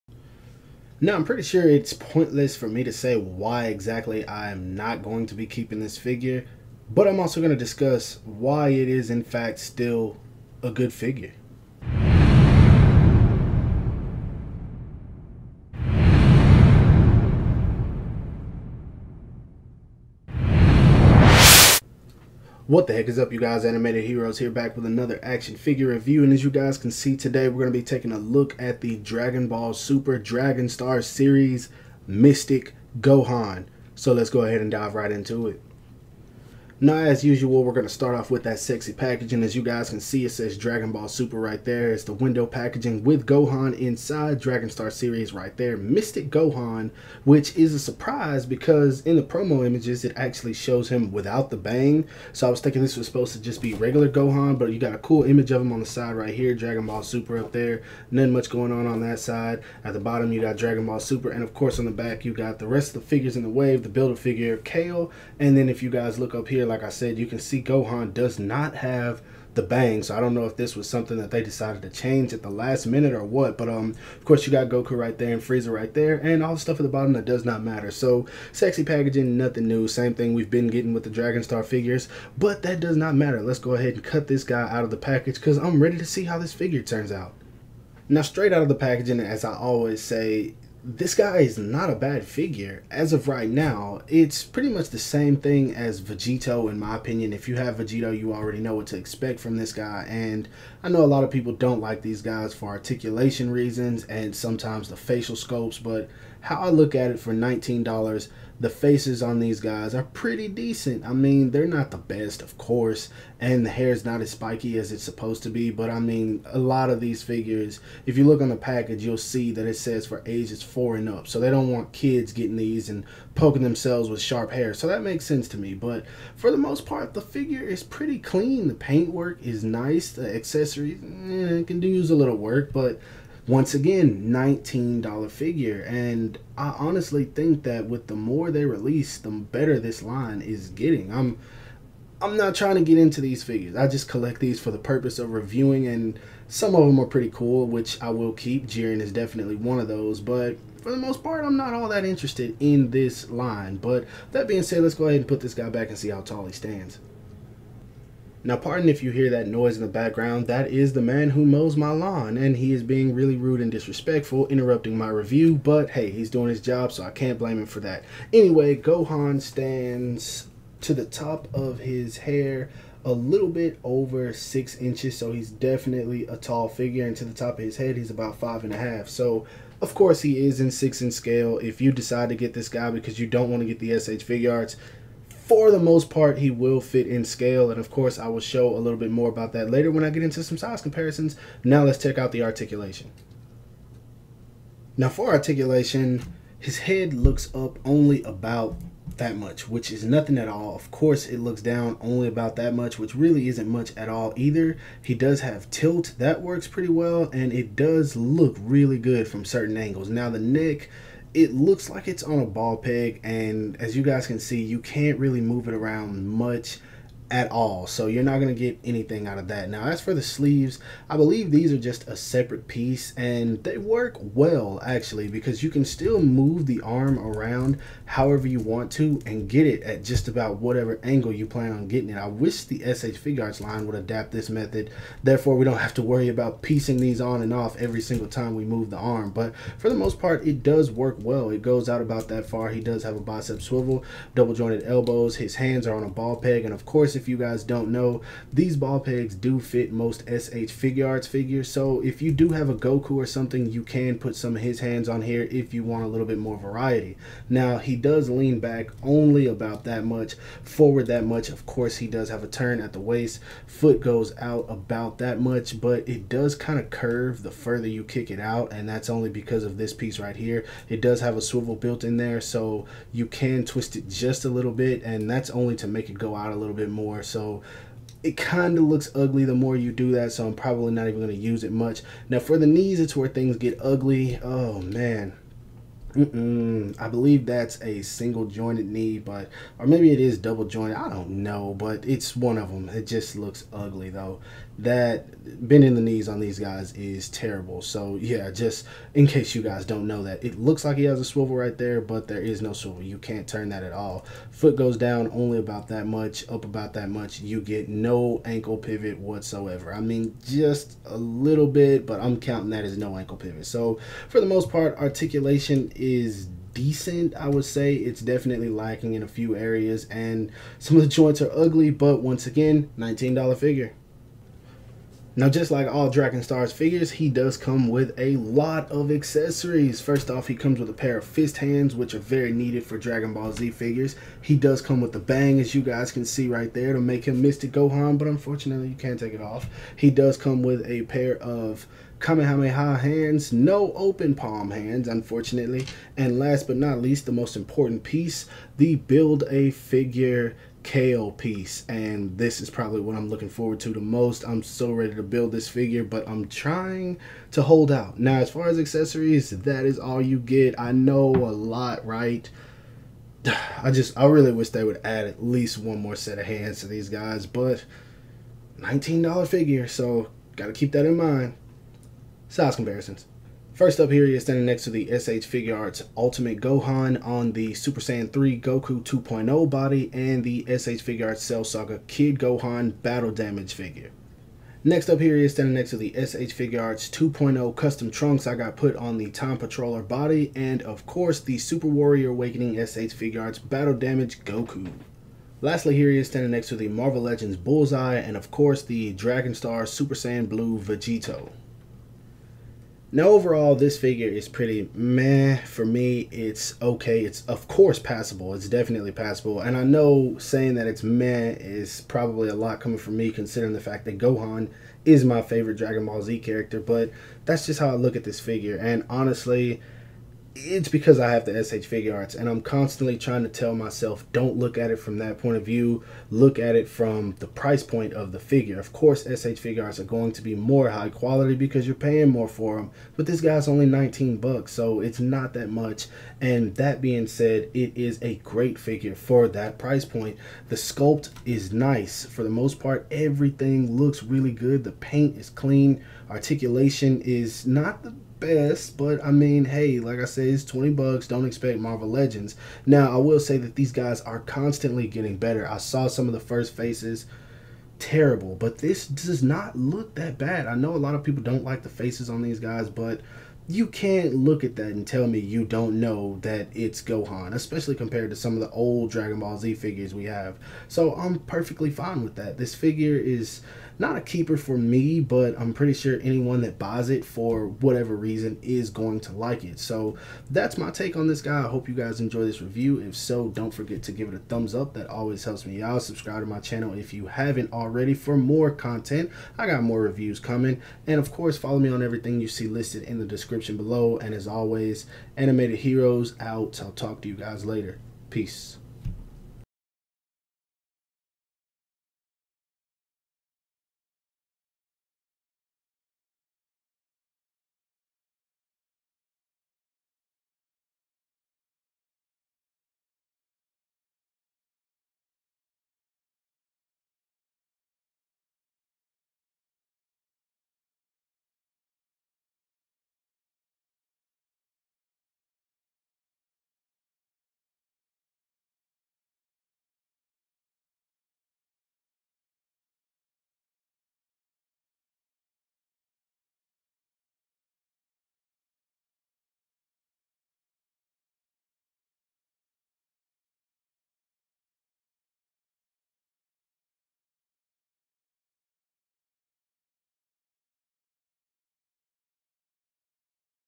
Dragon Stars Dragon Ball Super Tournament of Power Gohan Action Figure Review BANDAI America
To be honest I have to say that this Dragon Stars Dragon Ball Super Tournament Of Power Gohan Action Figure Is much better than I thought it was going to be. Truthfully I had no intentions on getting this figure. My only purpose for buying it was because I wanted to complete the build-a-figure Kale. Now I am almost certain I will probably keep this action figure (for a little while) in my collection. As we all know Gohan is a fan favorite so I highly recommend this figure if you don’t want to spend an insane amount on the Web Exclusive Version that releases from SH Figuarts.